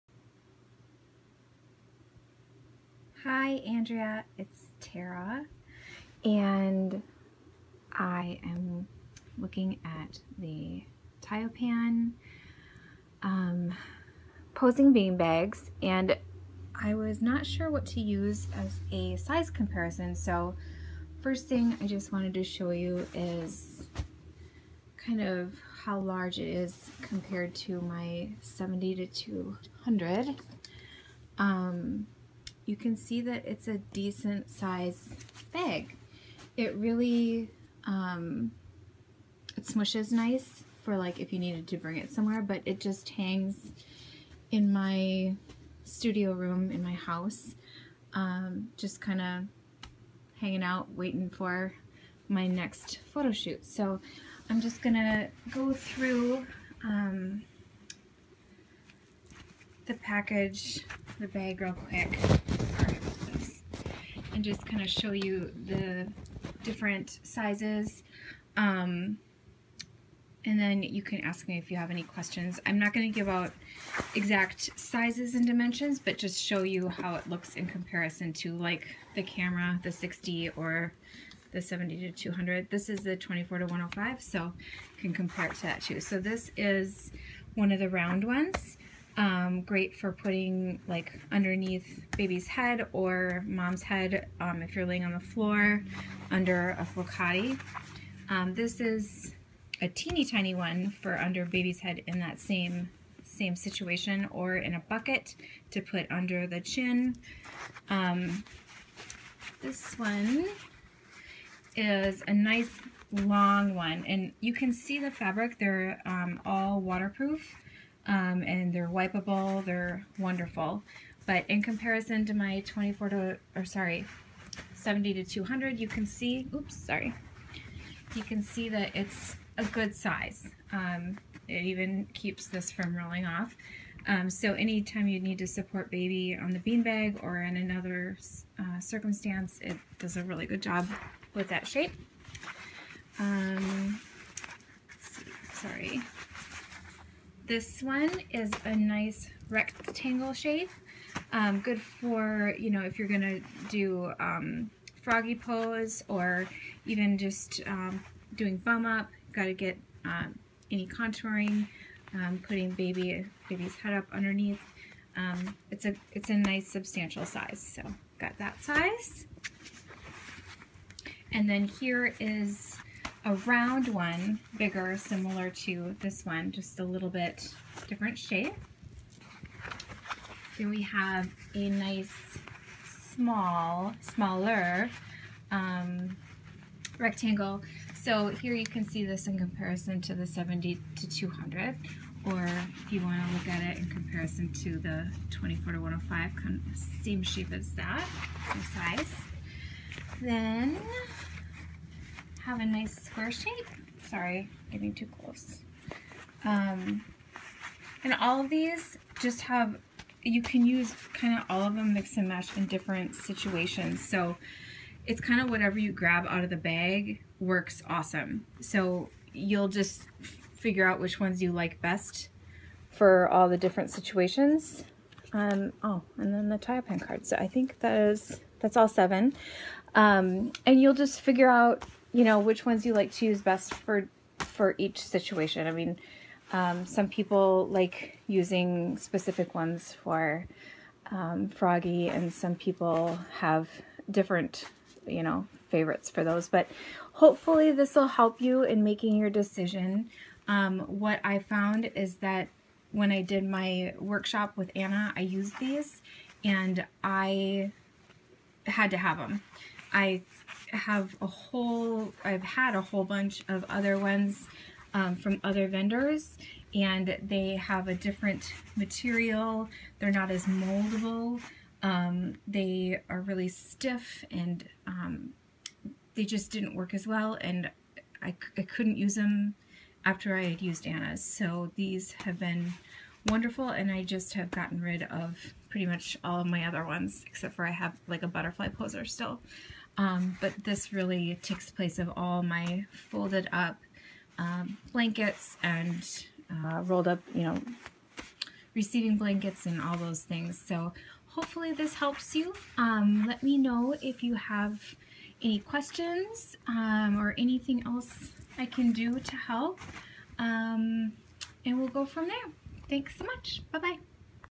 TAoPaN Posing Bean Bags
An up close look at the TAoPaN bean bags for posing newborn babies during photography sessions.  They are really great!